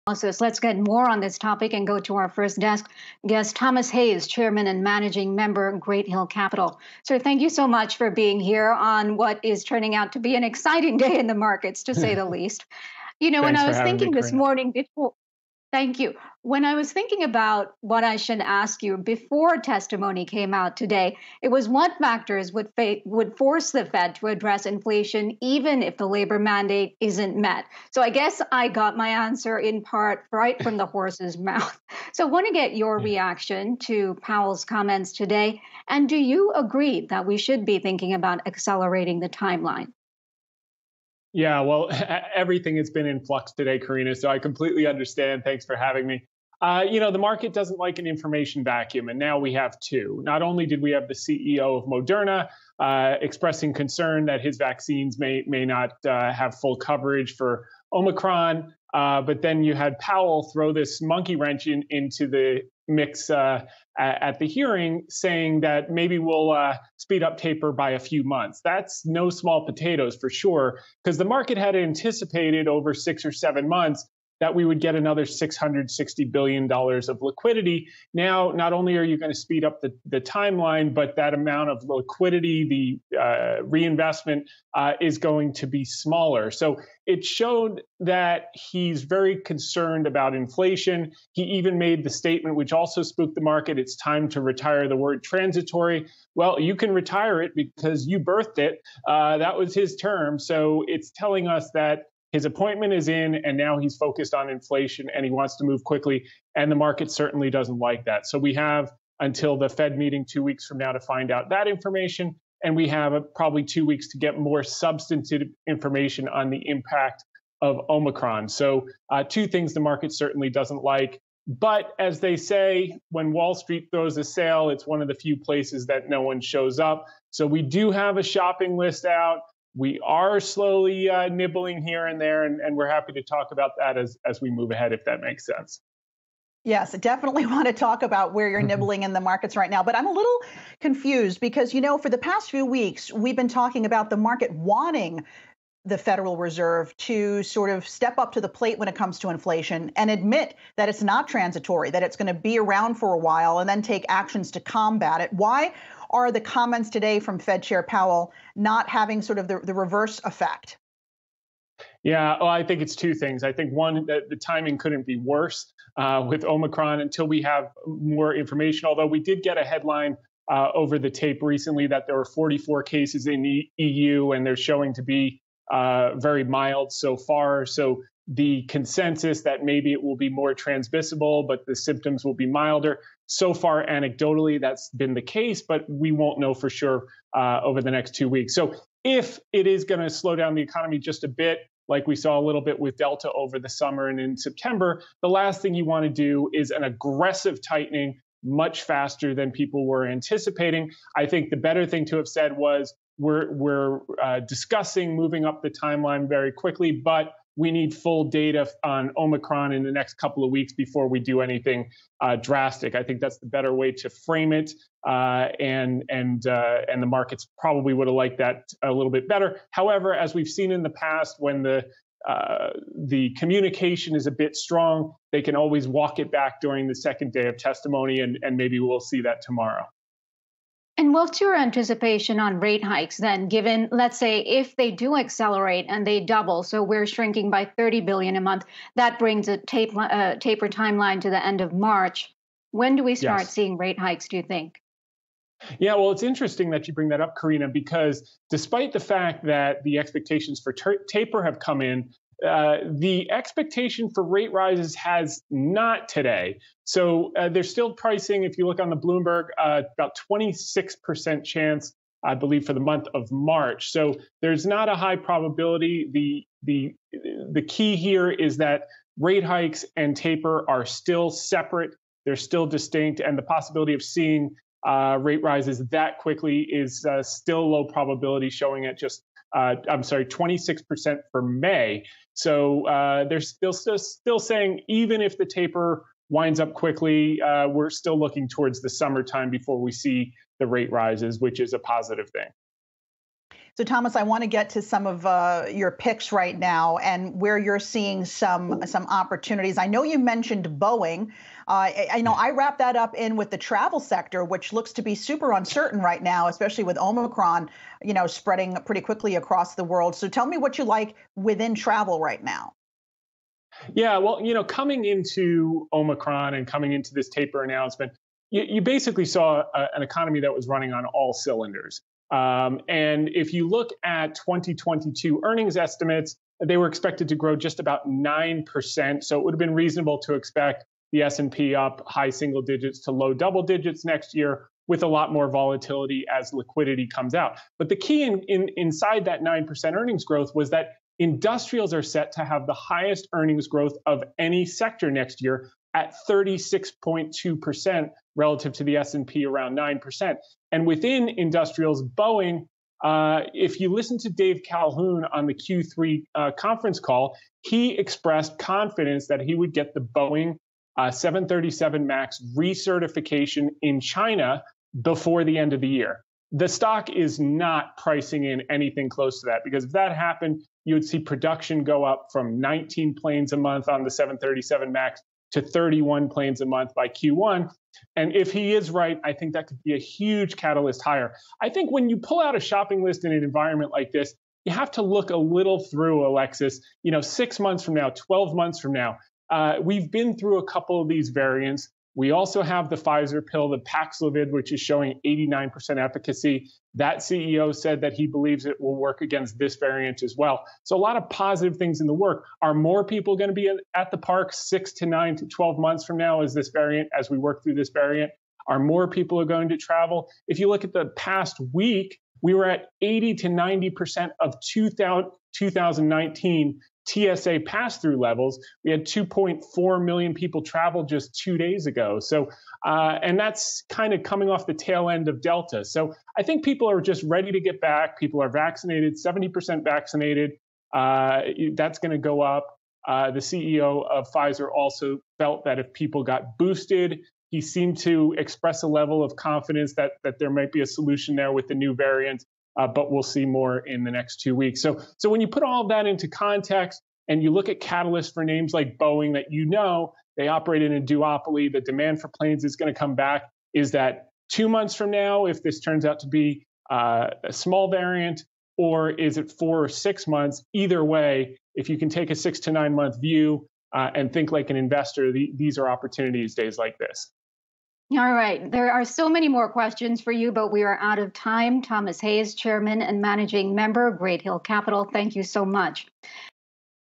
Tom Hayes - Yahoo! Finance Appearance - 11/30/2021
Yahoo! Finance Appearance - Thomas Hayes - Chairman of Great Hill Capital - November 30, 2021